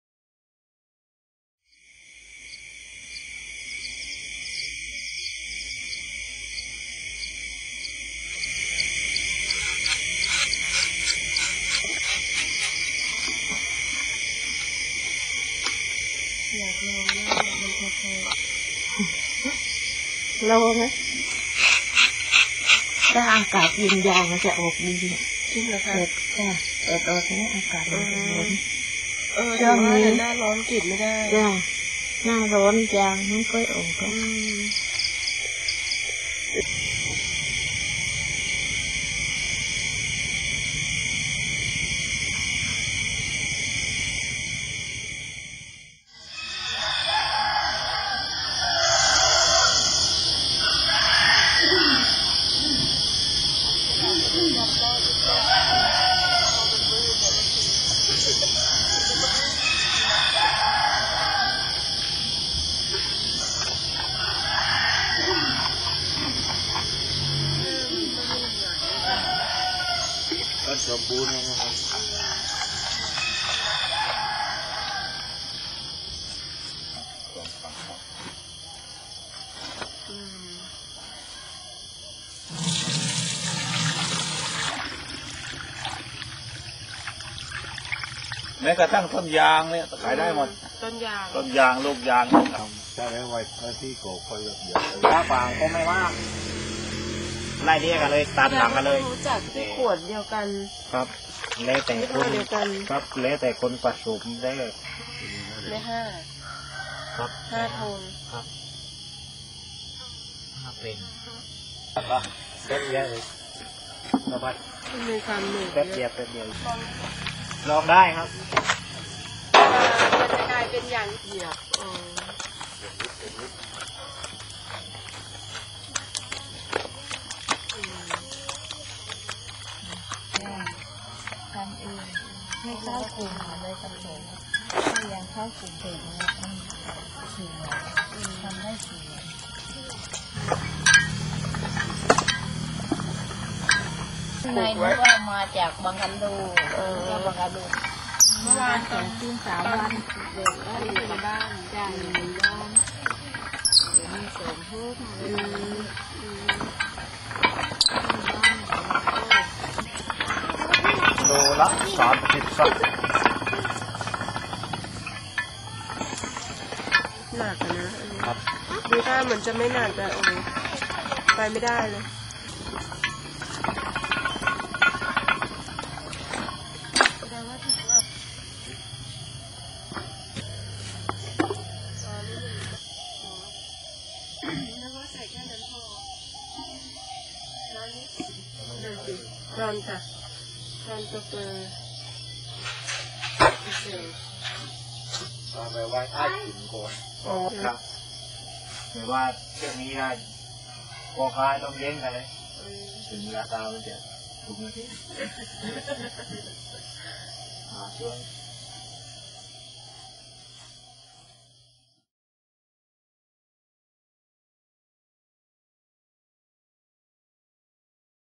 The Elastic Forest - (making of natural rubber)
All the process involved in making ribbed sheets of Natural Rubber